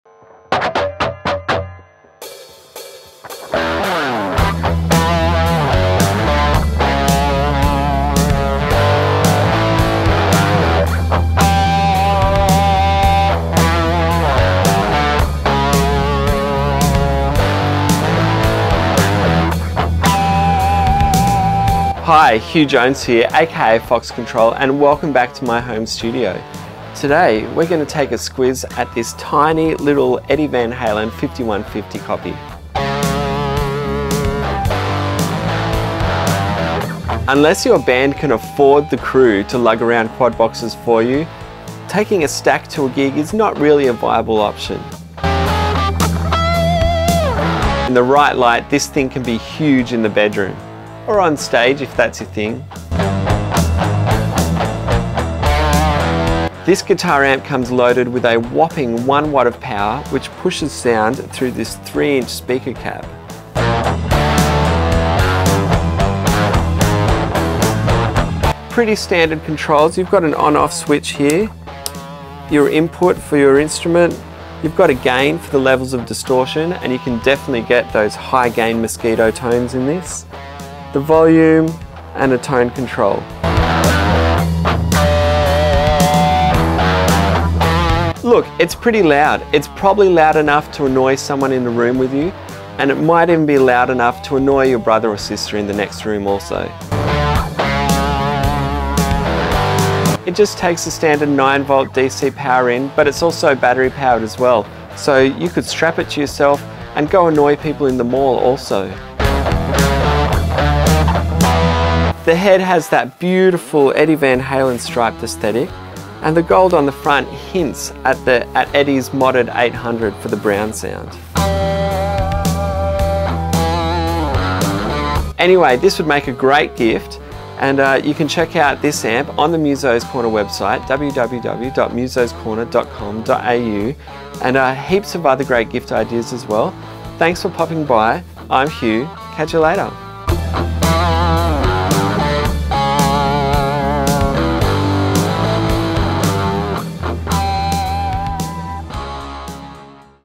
Tiny amp, huge sound!!!
This is the most compact way to sound like Eddie Van Halen.

Just like it’s bigger brothers, the EVH® 5150III® Micro Stack is filled with arena-sized crunch—yet you won’t need a road crew to move it. Perfect for practicing in a dorm room or office, it features the look and sound that put EVH amplifiers on the map. With a full set of Gain, Volume and Tone controls you can dial in your sound as clean or gritty as you want. It even includes a headphone output jack for silent practice.

Features:
• One-watt single-channel amplifier
• Single 3” speaker
• Dedicated gain, volume and tone controls
• Integrated tilt-back kickstand
• ¼” headphone jack and 9VDC adapter jack
• Dimensions:
o Height: 6.92”
o Width: 5.35”
o Depth: 2.63”